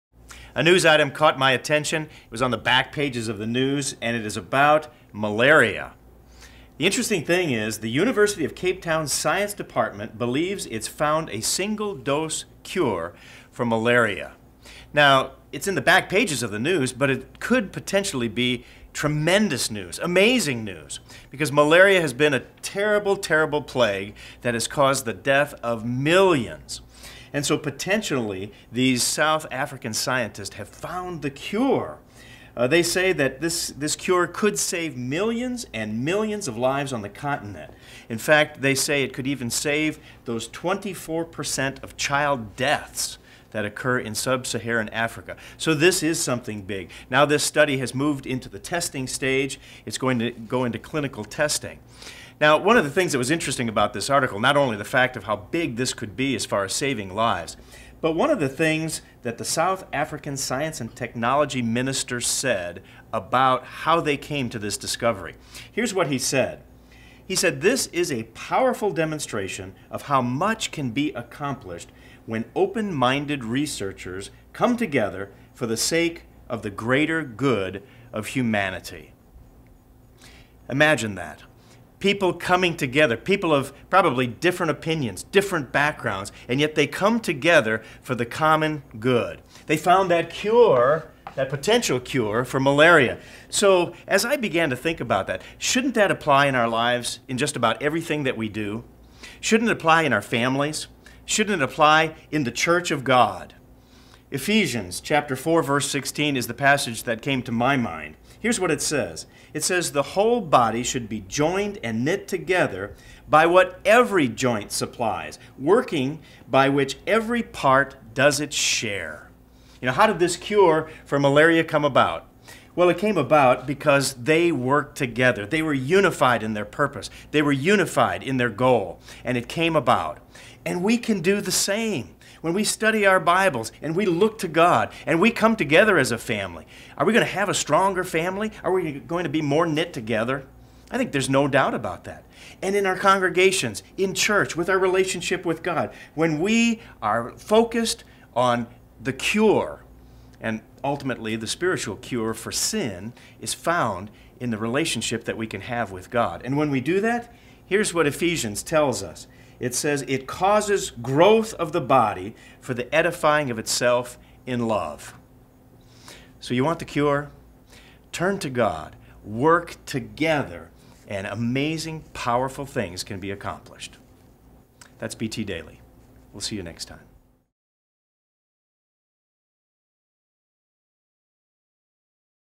BT Daily: Lessons in a Cure for Malaria
September 4, 2012 - South African scientists may have a cure for malaria. How did they do it? Is there a lesson to learn?